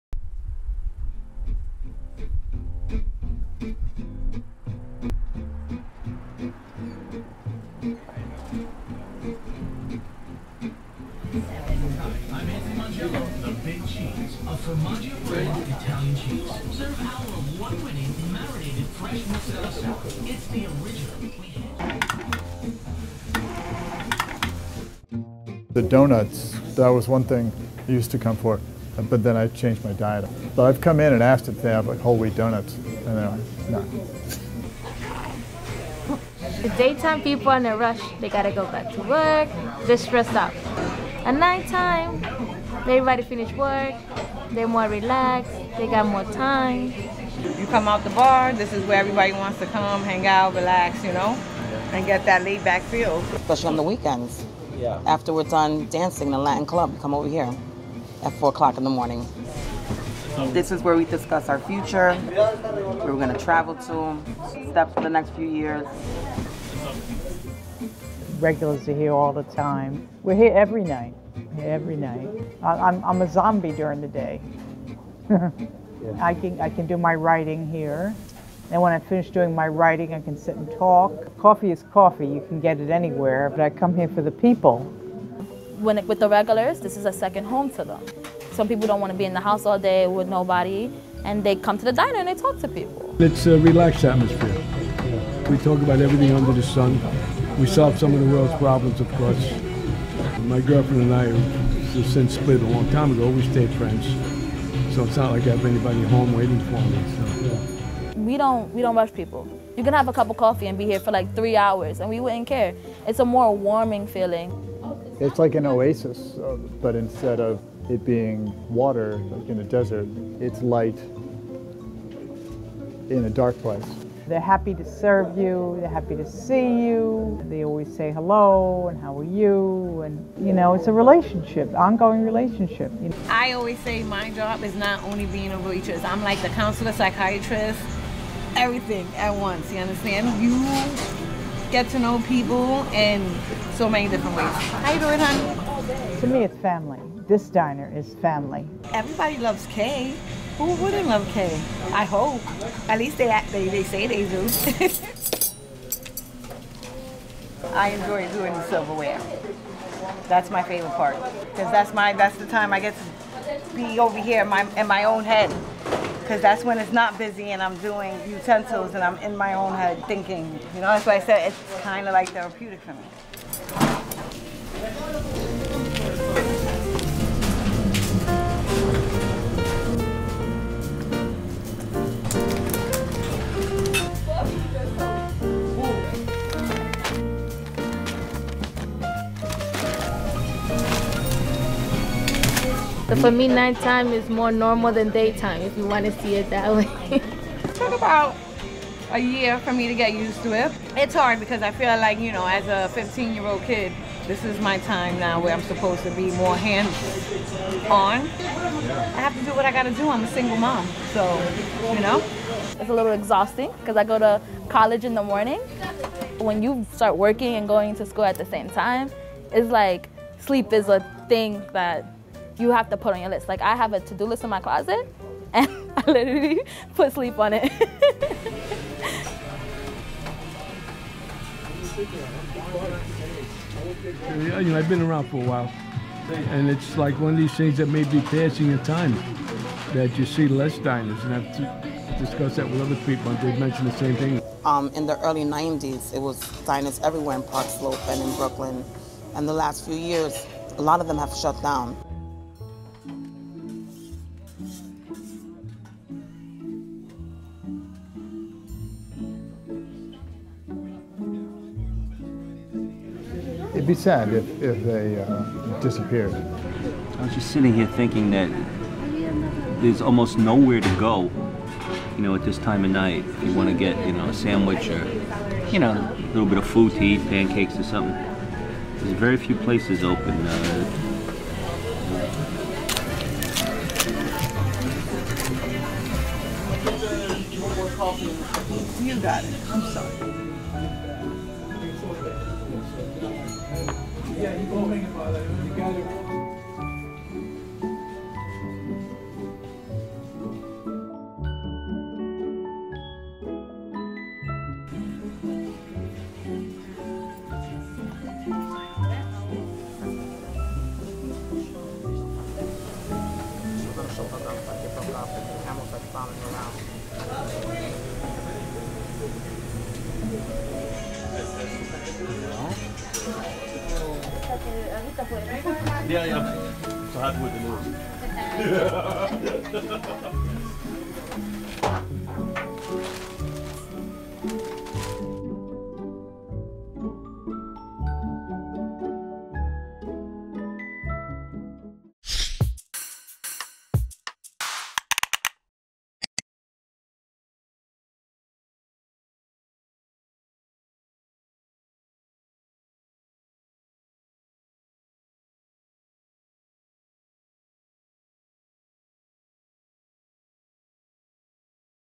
Donuts After Dark | BK Stories
7th Ave Donuts and Diner has been serving Park Slope since 1977. Open 24/7, the diner has a loyal daytime and nighttime crowd and a welcoming atmosphere for both the regulars and staff. With the number of diners and luncheonettes in the city steadily declining, the 7th Ave Donuts and Diner family show us exactly why it's so wonderful to be able to get a fresh doughnut at any hour.